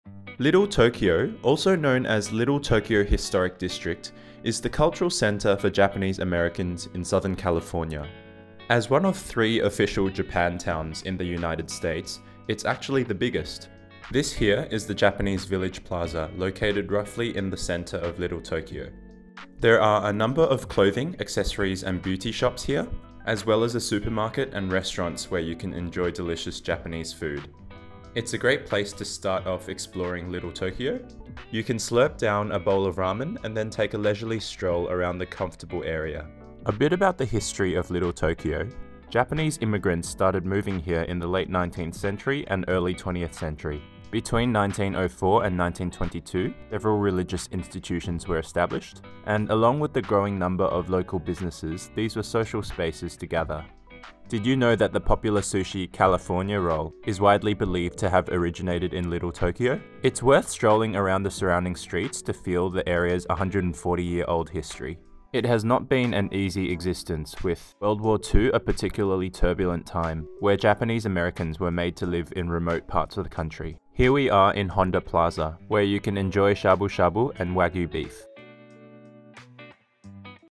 Walking around Little Tokyo Historic District, the BIGGEST Japan town in the United States
Little Tokyo, otherwise known as Little Tokyo Historic District, is the biggest one of three Japan towns in the United States. It's also one of the oldest areas in Los Angeles, with a 140-year history dating back to the late 19th century.

There are plenty of dining options here, where you can enjoy a bowl of ramen, shabu-shabu hot pot, sushi, and other umami Japanese cuisine.

The Japanese Village Plaza is located in the centre of Little Tokyo, and is a great area to start exploring the community. With its lantern decorations, traditional architecture and shops, there is plenty to see.

Walking around the streets of Little Tokyo, you will find restaurants, cultural centres, religious buildings, and hotels. The cafe inside Miyako Hotel is cozy and nice to catch-up in. Honda Plaza is where you will find Tensho, a premium Japanese hot pot restaurant serving king crab and mouthwatering beef.